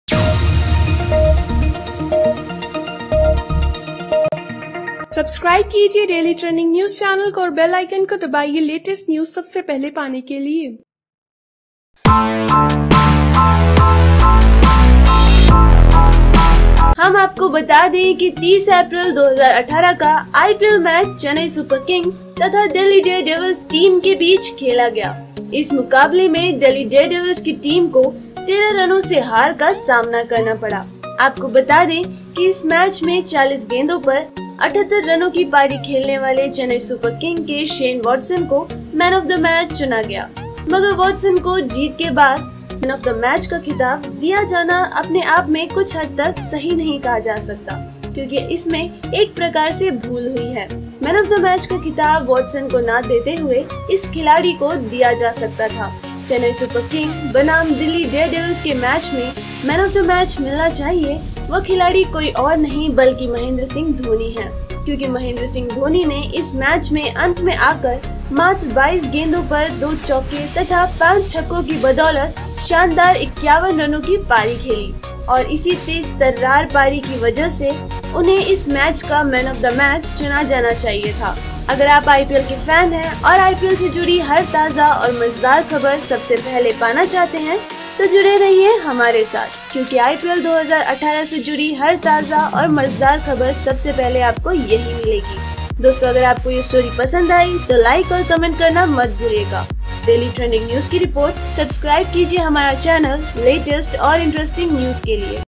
VIVO IPL 2018 - CSK vs DD: मैन ऑफ द मैच देने में हुई बड़ी भूल, वॉटसन नहीं ये खिलाड़ी था असली दावेदार
Edited By --Jiya Chauhan